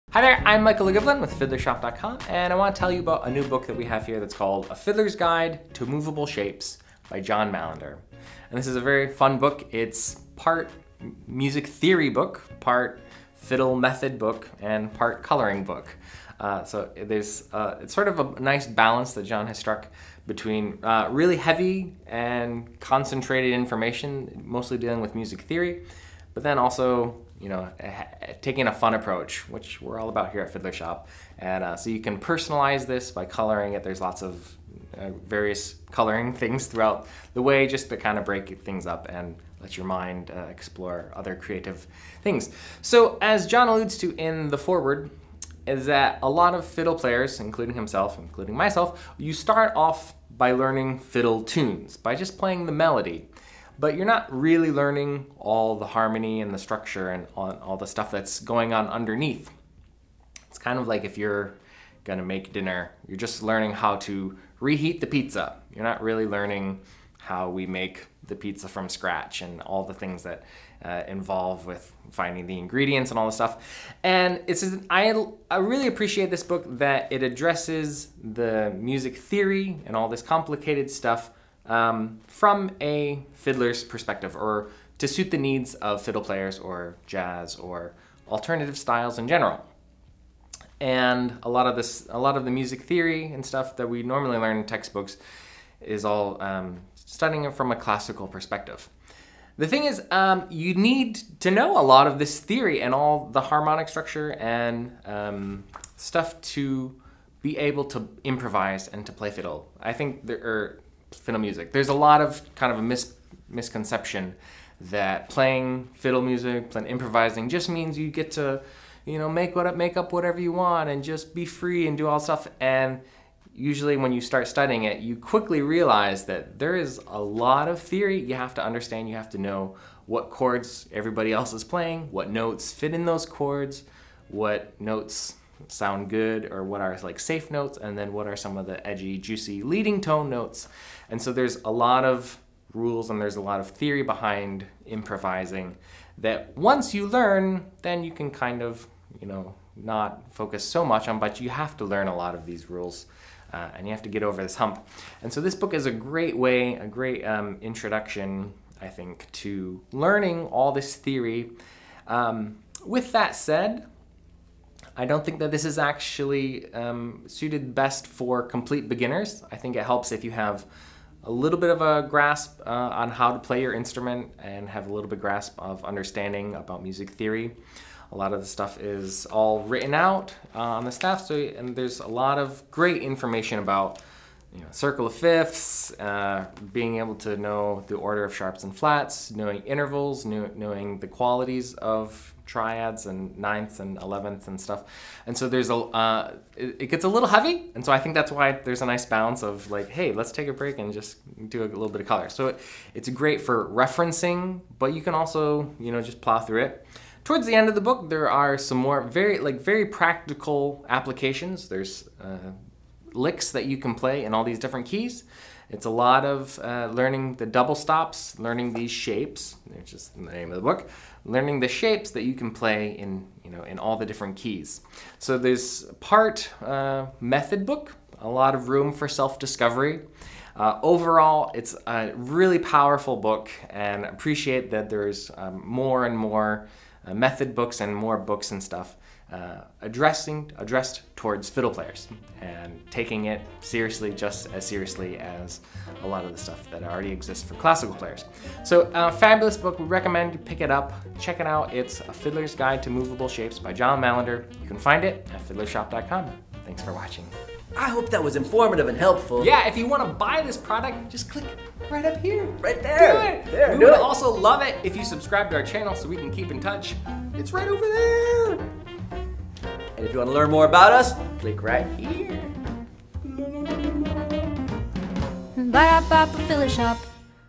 A Fiddler's Guide to Moveable Shapes - Review
A Fiddler's Guide to Moveable Shapes is the world's first fiddle-focused chord shape coloring book for visually-inclined, harmonically-curious string players of all levels. From the mind of acclaimed fiddler John Mailander, the guide combines traditional music notation with striking visuals for a unique learning experience. Colored pencils included.